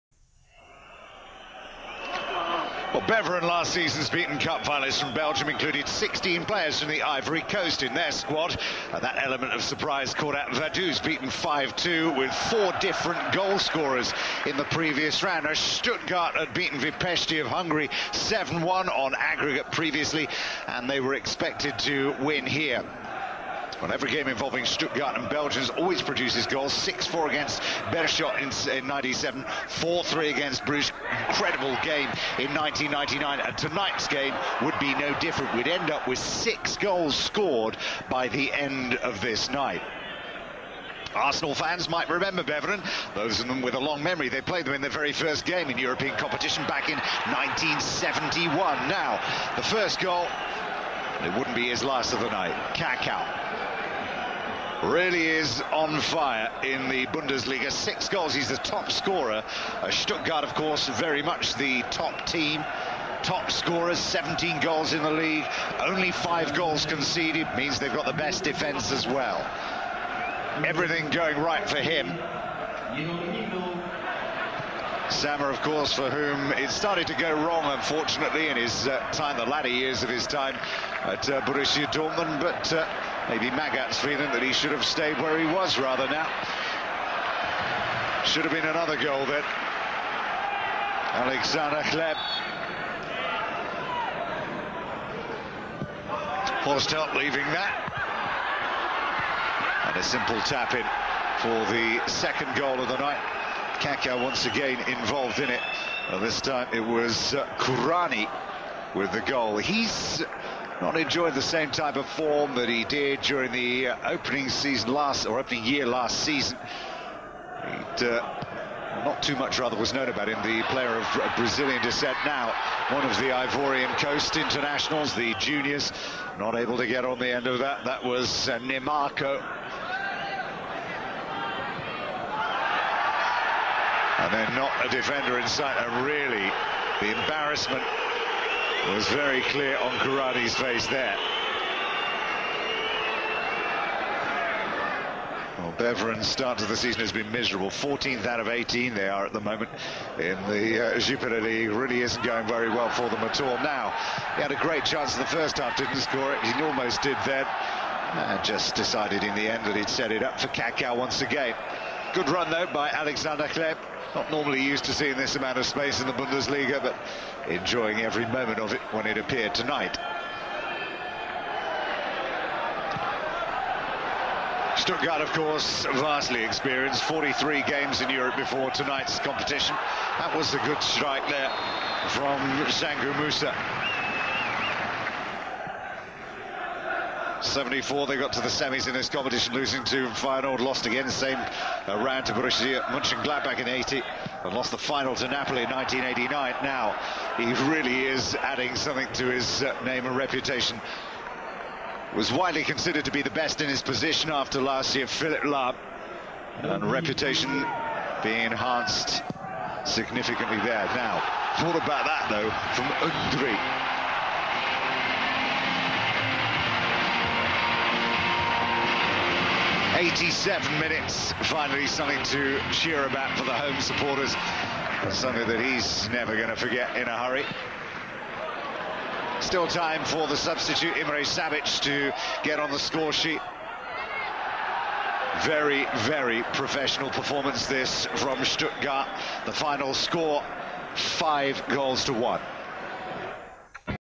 Beveren 1-5 Stuttgart (21.10.2004)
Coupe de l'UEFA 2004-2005
Groupe G ; 1ère journée

Buts :
9' Cacau (Stuttgart)
39' K.Kuranyi (Stuttgart)
52' Cacau (Stuttgart)
76' P.Lahm (Stuttgart)
86' Romaric (Beveren)
90' I.Szabics (Stuttgart)